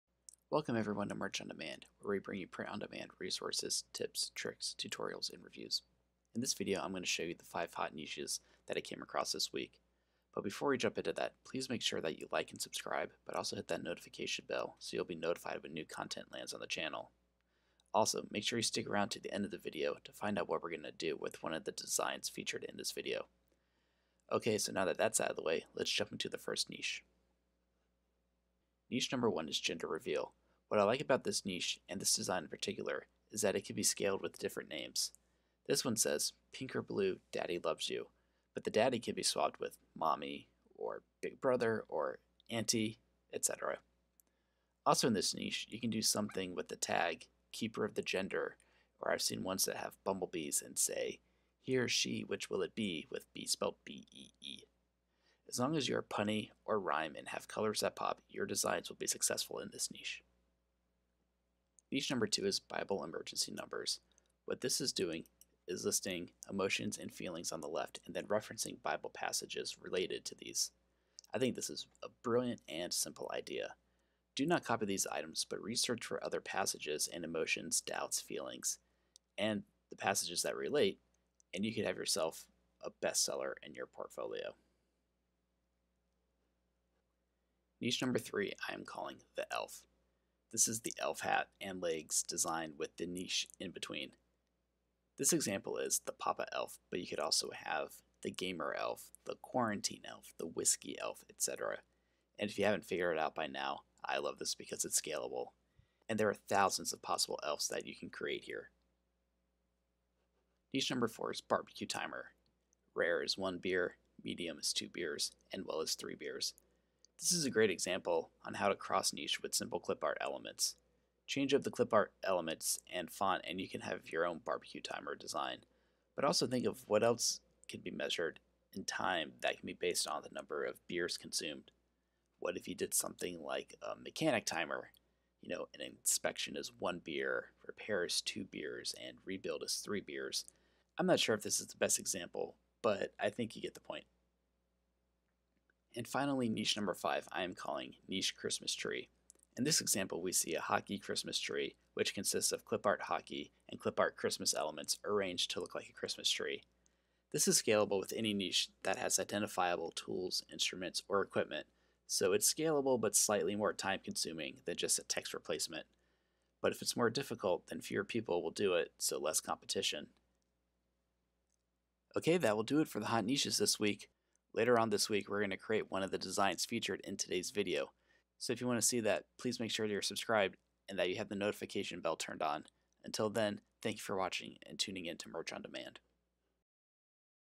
Hot Niches of the Week - Print On Demand Niche Research #2
These are HOT niches this week on Merch by Amazon.  Hopefully you will find something new here and be inspired to make your own designs!